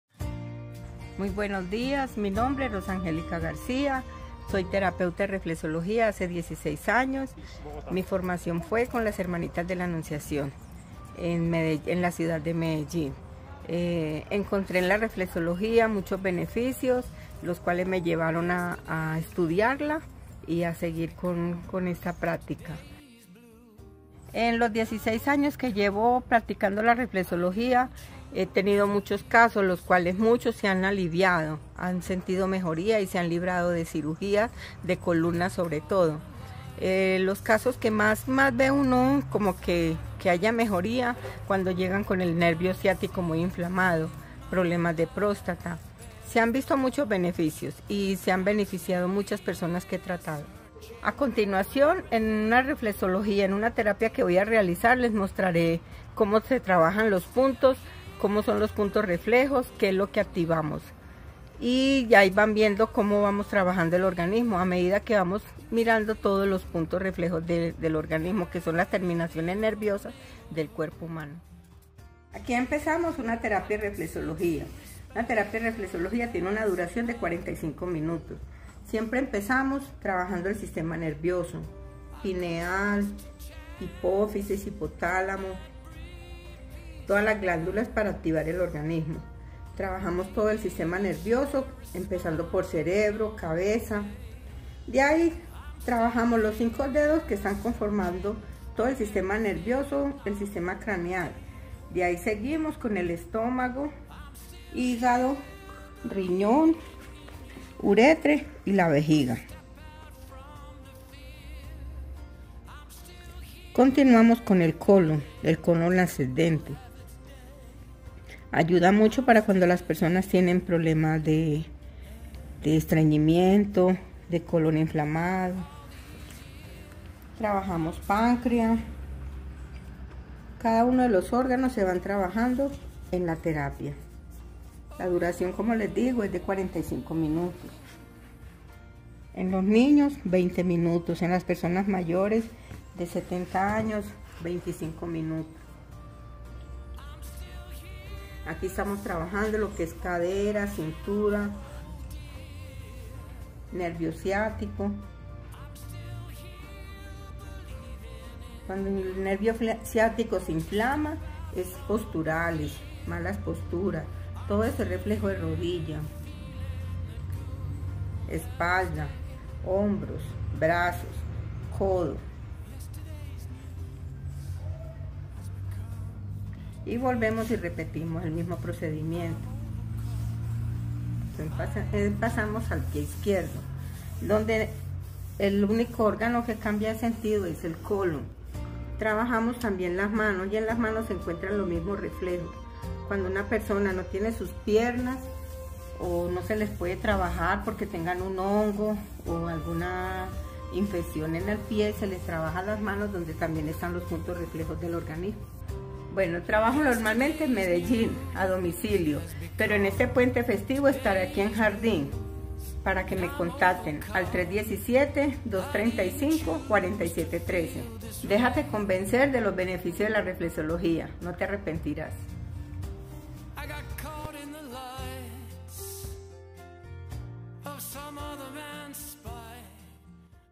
Encuentra en la reflexología un alivio para muchos problemas.
Rosa García, es reflexóloga, es decir, hace terapias de reflexología, una medicina alternativa muy efectiva.  Ella atiende a domicilio en Medellín para que la contactes y, este puente festivo del 7 de Agosto (4 al 7), podrás encontrarla en Jardín haciendo unas terapias muy sanadoras y relajantes.

También hace masajes corporales.